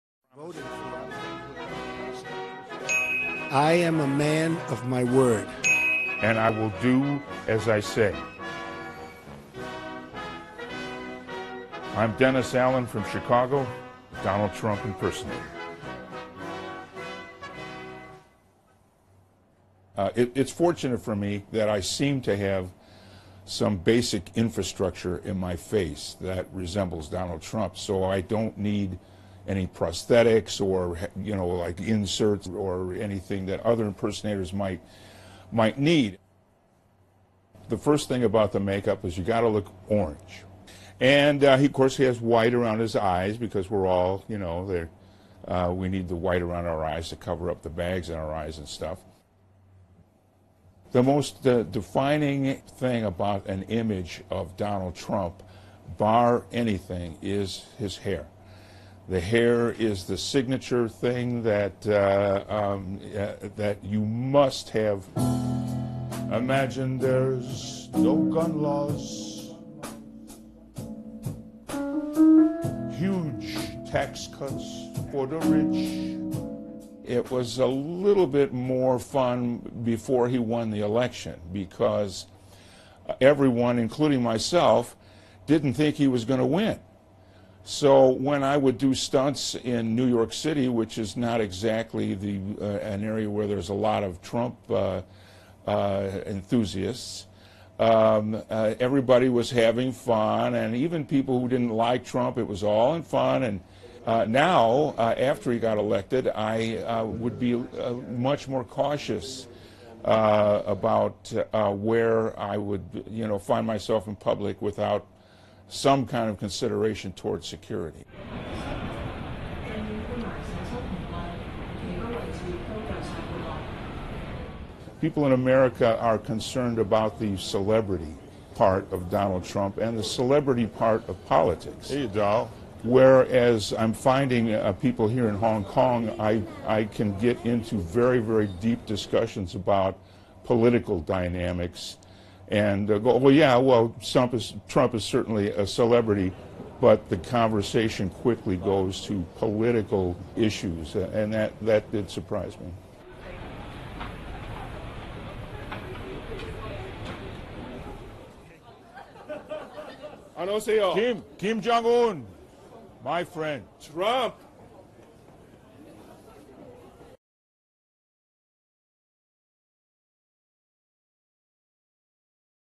Двойник Дональда Трампа
еннис Алан — двойник Дональда Трампа. Он говорит, что реакция публики на его выступления изменилась после того, как Трамп стал президентом